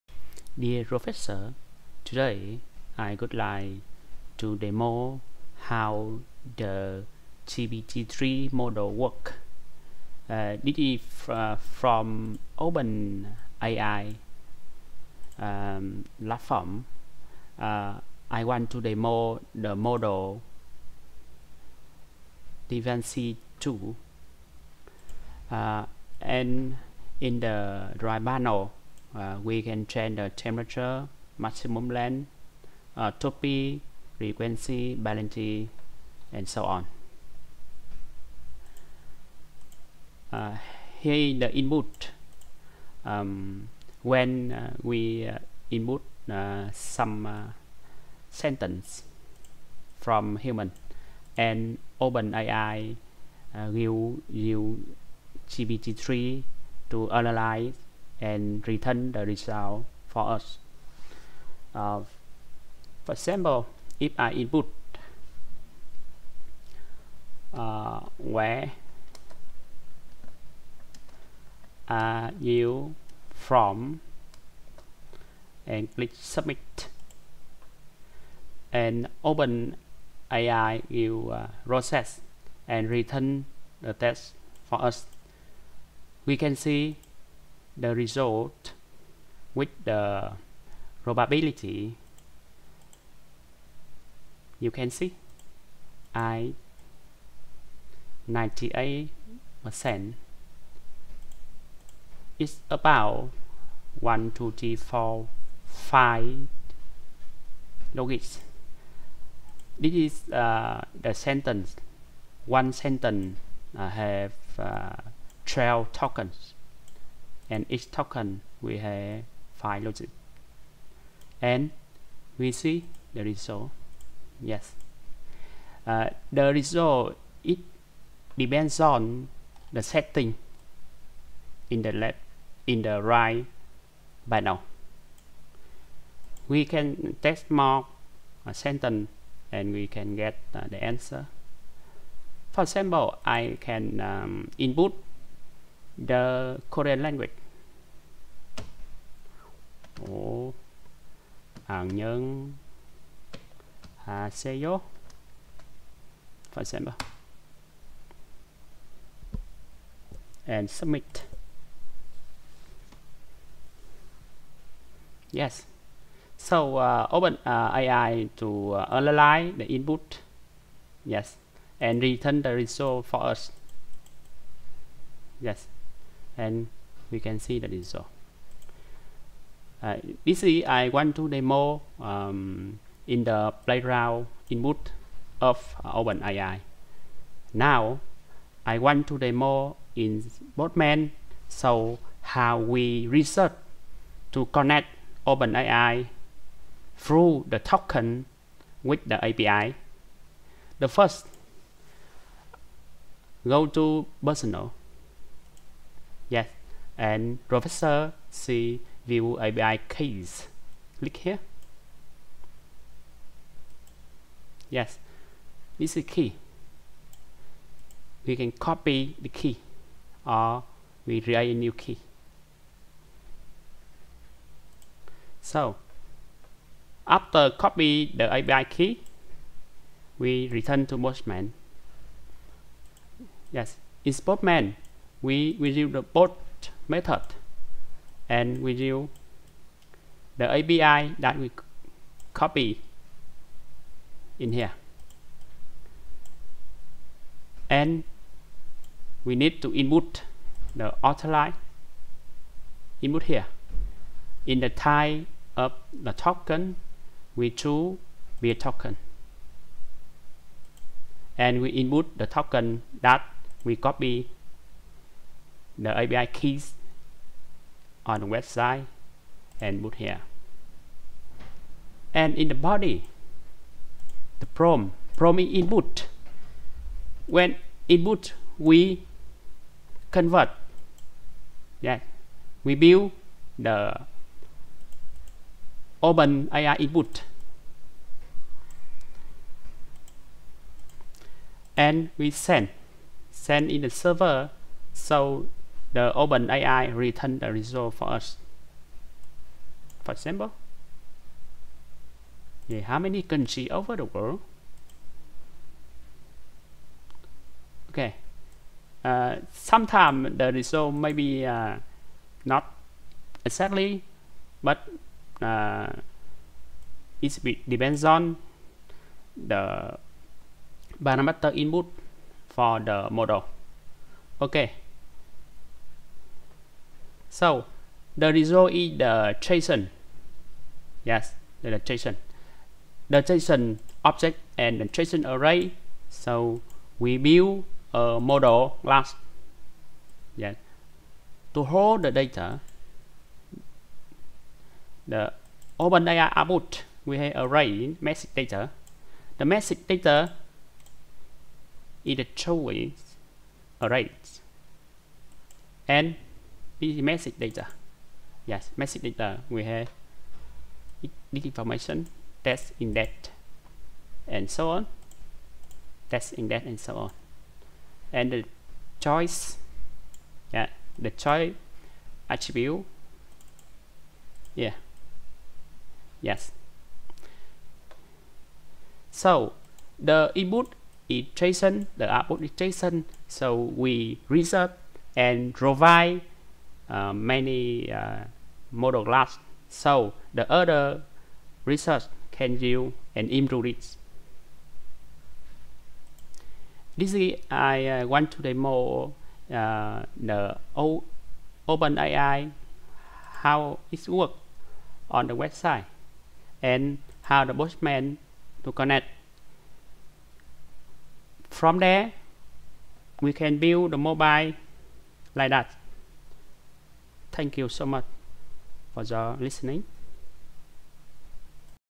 Demo Playground tool and Postman to interacts with OpenAI GPT 3 model
This demo is from Data Science Lab - KMOU.

The Authors: Duy Thanh Tran, Prof Jun-Ho Huh